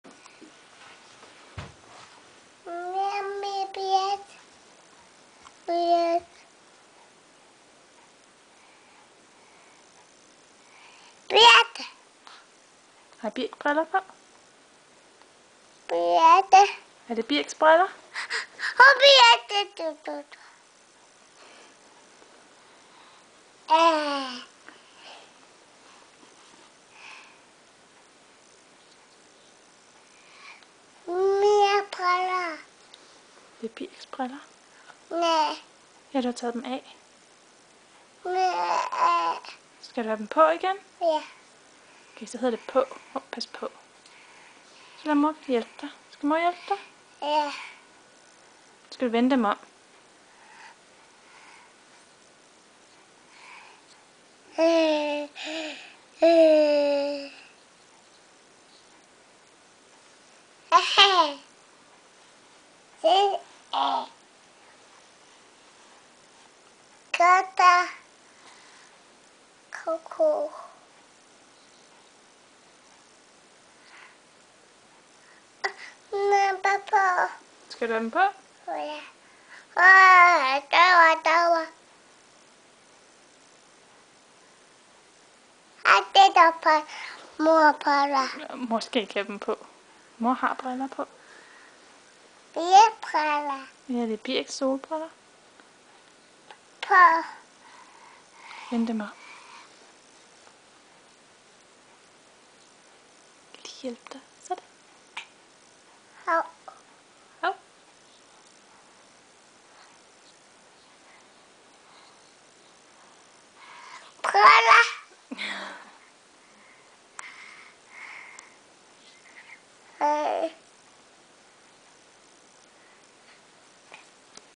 Birks solbriller
Birk er blevet glad for at iklæde sig forskellige hatte, strømper og solbriller. Her tester han solbriller.